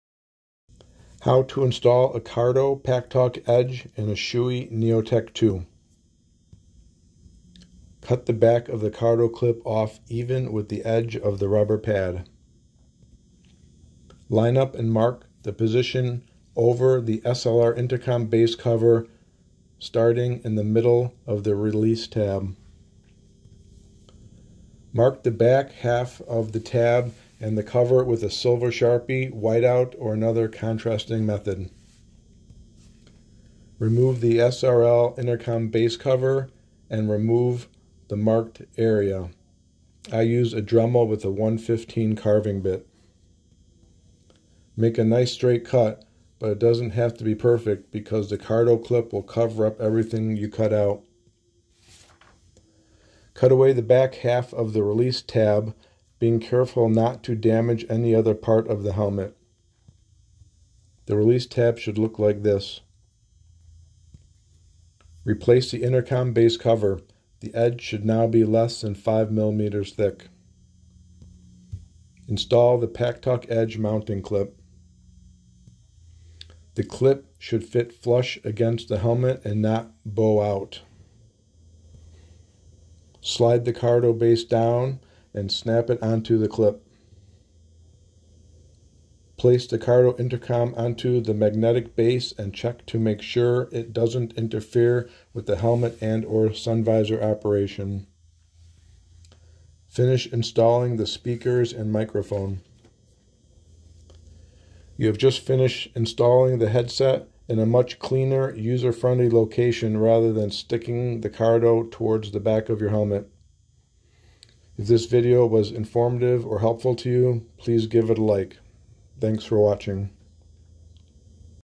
How to install a Cardo Packtalk Edge in a Shoei Neotec 2, Neotec II
Install a Cardo Packtalk Edge in a Shoei helmet designed for a Sena SLR headset.